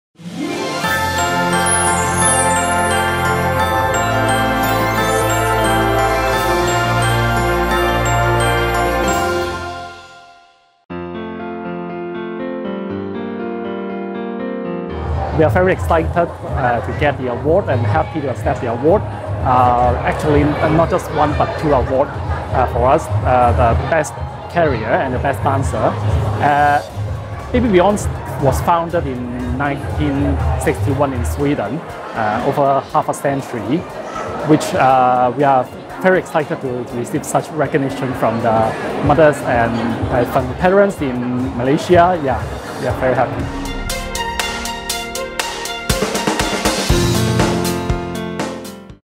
Parents' Choice Award Winner- BabyBjorn- Best Bouncer & Carrier
Congratulations to our award winner- BabyBjorn for winning two awards at our Parents' Choice Awards for the Best Bouncer & Best Carrier category!